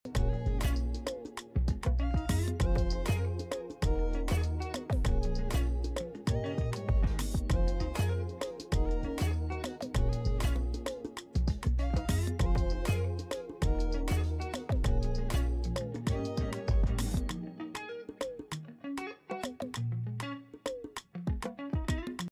Removedor Tiresmalt Impala - O Melhor removedor para esmalte escuro e glitter
Oi meus amores, Tem esmalte azul mais lindo que esse? NÃO, não tem!
Cor Royal @mundial_impala
Usei o removedor Tiresmalt para cores escuras e glitter. Ele realmente cumpre o que promete, não tive dificuldade nenhuma para limpar os cantinhos. @mundialbeautycare

Não esqueçam de se inscrever, curtir o vídeo e compartilhar com suas amigas.